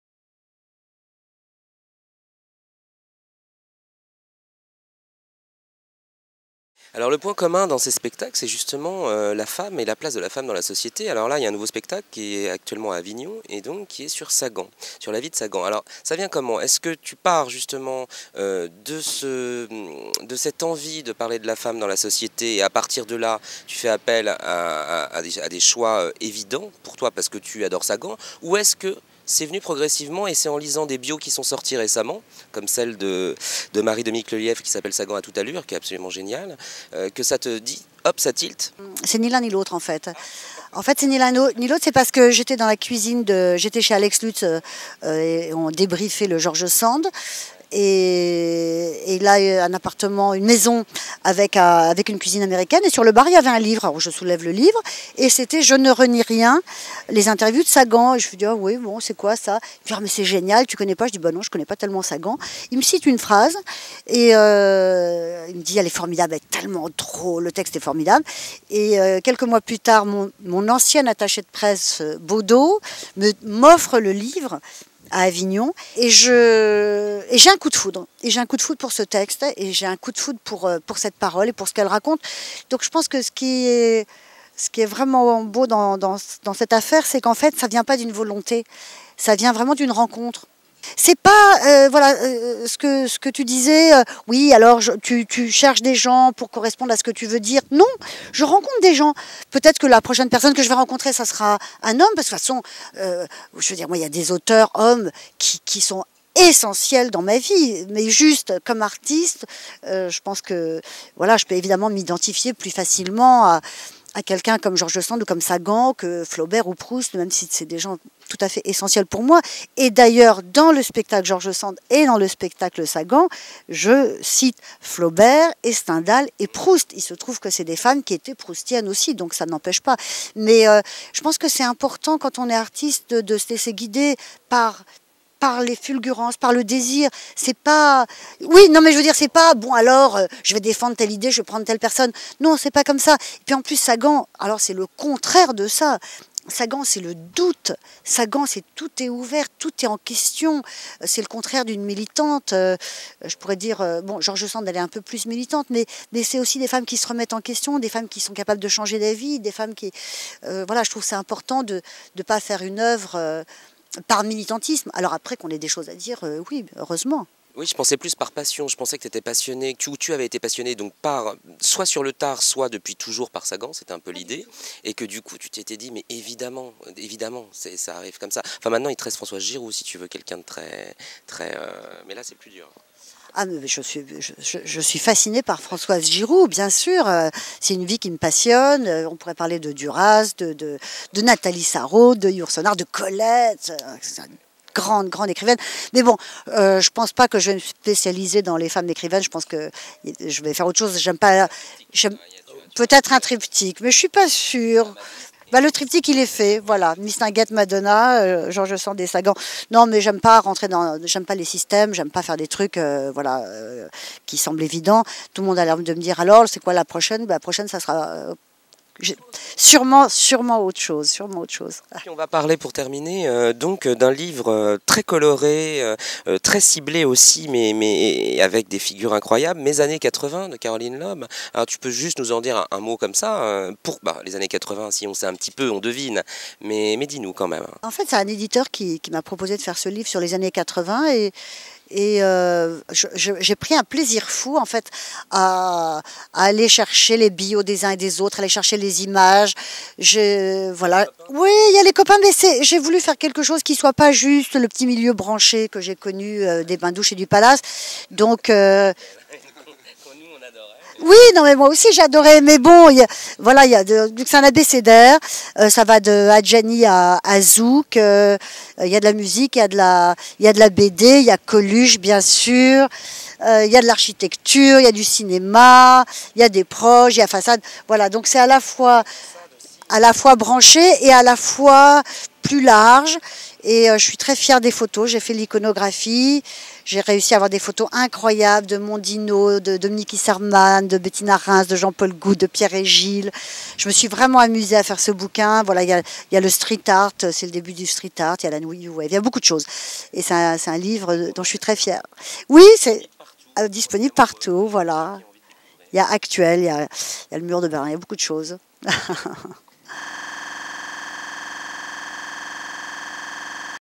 Caroline Loeb et Françoise Sagan. Les années 80
Caroline LOEB en interview dans l'Œil du Spectacle. A propos du spectacle "Françoise par Sagan". Entretien avec Vincent Gramain réalisé lors des Francofolies de la Rochelle en juillet 2016.